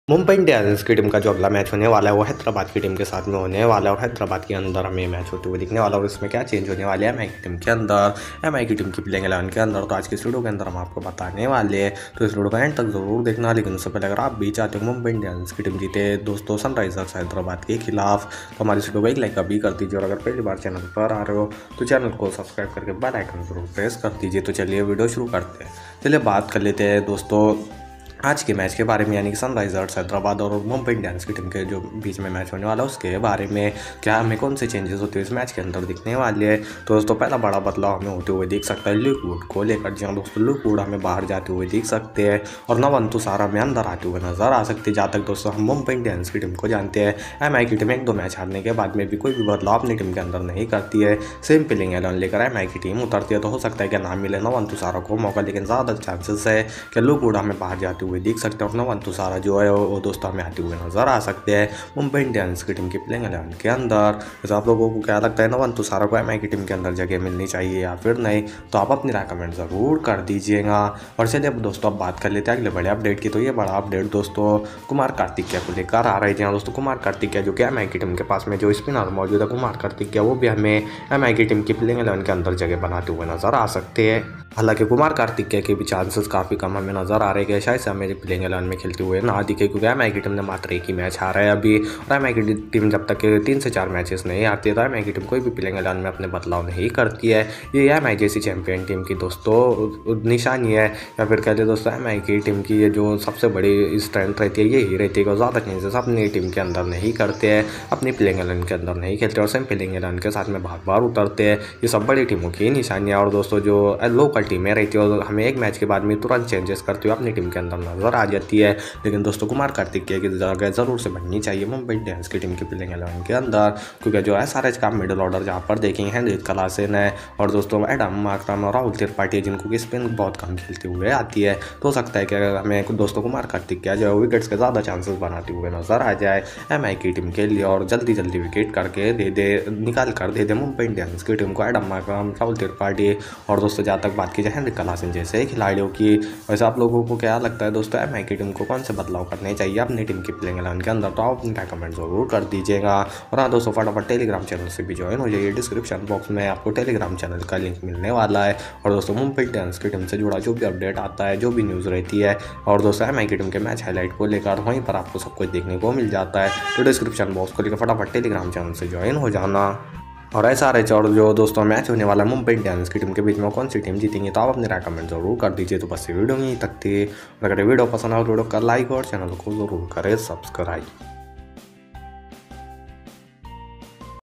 IPL 2024 - 3 Big Changes In Mumbai Indians Playing 11 | MI vs SRH 2024 | Only On Cricket
THIS VIDEO RELATED FOR  IPL 2024 AUCTION STRATEGY & TARGET PLAYERS